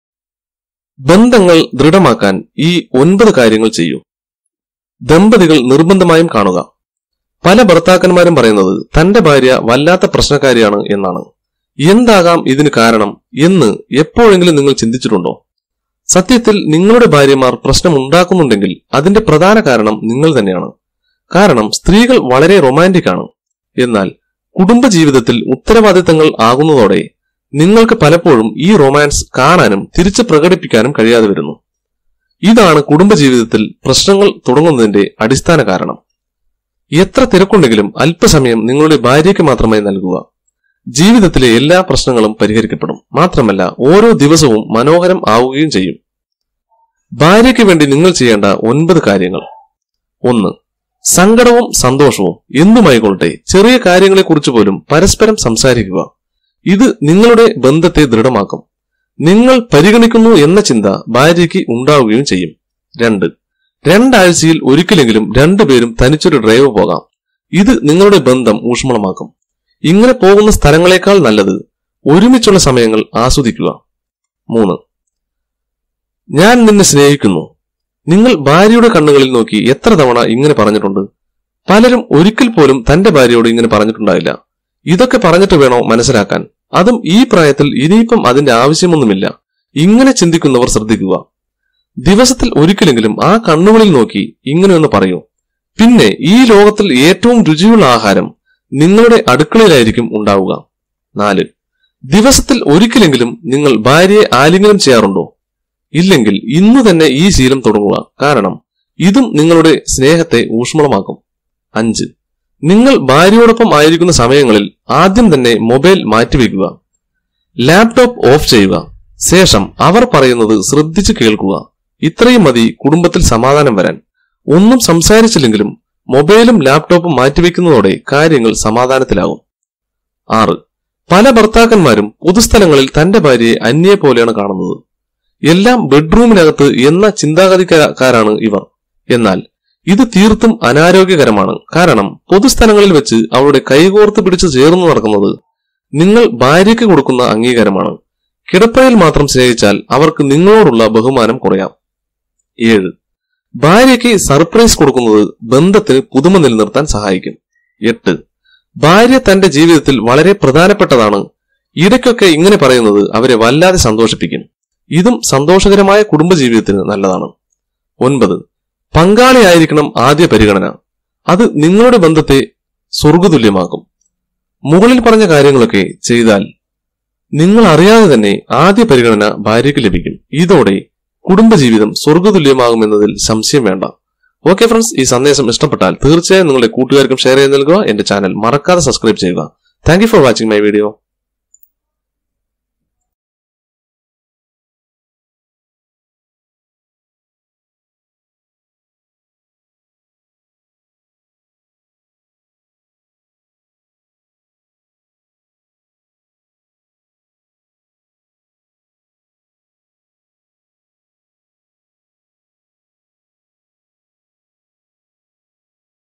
ബന്ധങ്ങൾ ദൃഢമാക്കാൻ ഈ 9 കാര്യങ്ങൾ ചെയ്യൂ | How Do You Change Your Life Better
How To Make Love | പല ഭര്‍ത്താക്കാന്മാരും പറയുന്നത് തന്റെ ഭാര്യ വല്ലാത്ത പ്രശ്‌നക്കാരിയാണെന്നാണ്. എന്താകാം ഇതിന് കാരണം. എപ്പോാഴെങ്കിലും ചിന്തിച്ചിട്ടുണ്ടോ.. സത്യത്തില്‍ നിങ്ങളുടെ ഭാര്യമാര്‍ പ്രശ്‌നക്കാരാകുന്നുണ്ടെങ്കില്‍ അതിന്റെ പ്രധാനകാരണം നിങ്ങള്‍ തന്നെയാണ്.